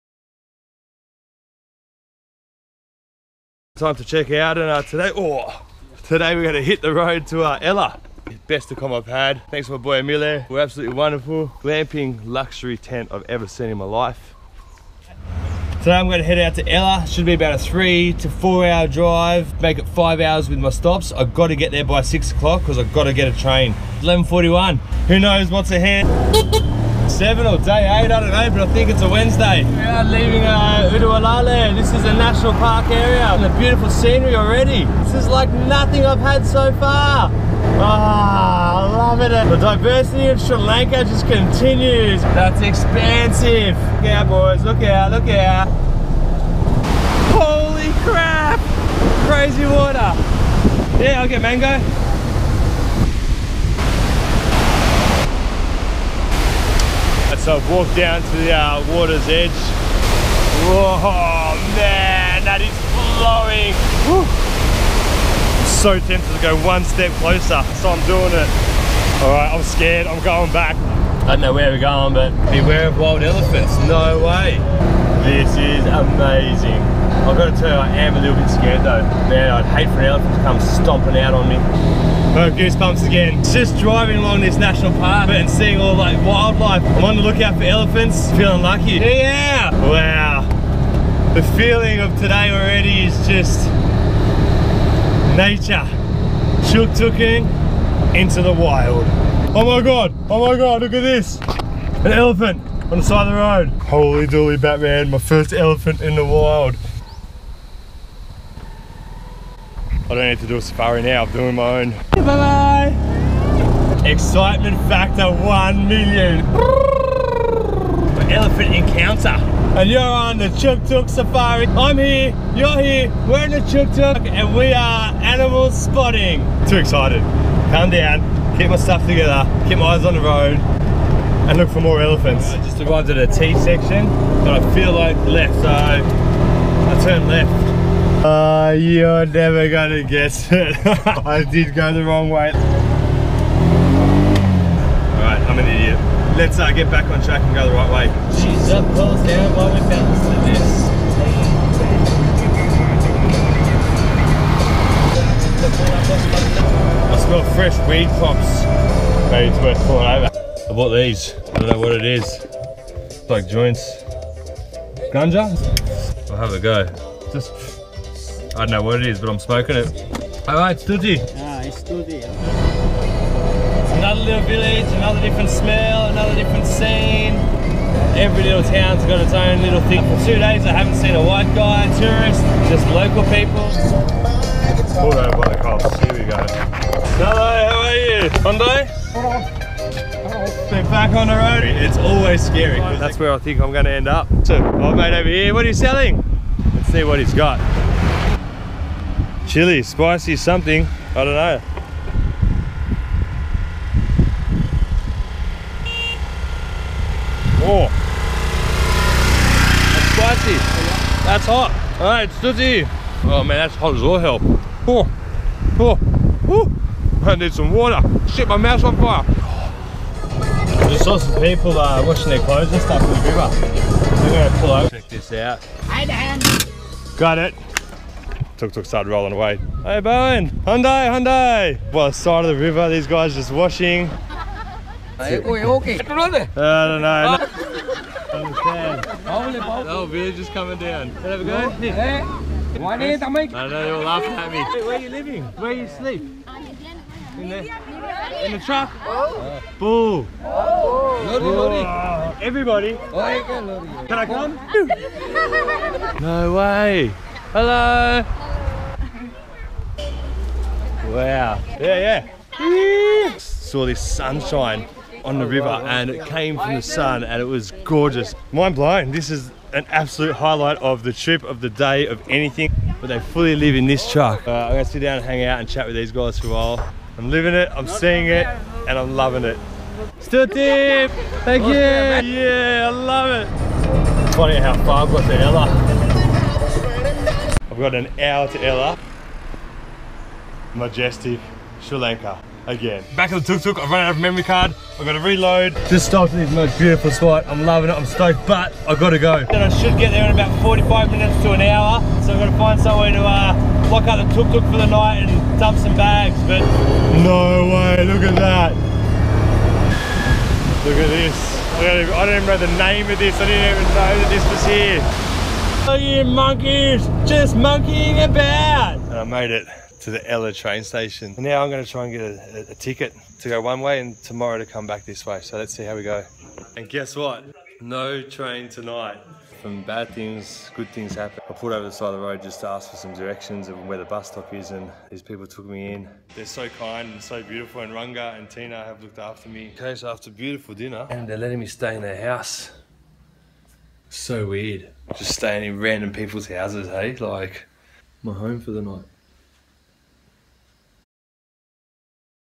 Chook Tuk Sri Lanka Ep 7 | Nature, Wildlife, Elephants,Waterfalls & Gypsies | Go Pro
Udawalawe- Reservoir- Ravana Falls- Ella

After waking up in the most majestic jungle tent, I leave Udawalawe heading for Ella Train Station. Today, I have my 1st elephant encounter, hang out with gypsies, burn my mouth, almost get washed away & smoke some……mmmmmm….?

Anyways, I only have 6 hours to get 110km to Ella- will I make it?
Live, LIKE, laugh, comment & SHARE in the journey...

More info, behind the scenes pics & stories on the official ChookTuk Sri Lanka page!